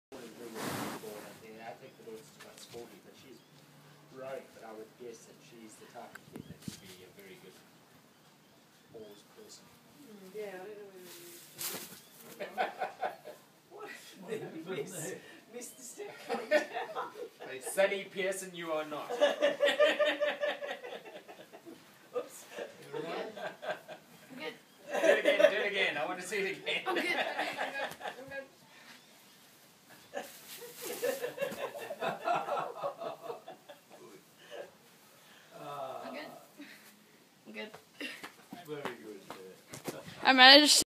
Parkour Fail
I saw joeys video and tried to have a go of Parkour on my own. Didn't really work, but at least I have a video of fail to call my own.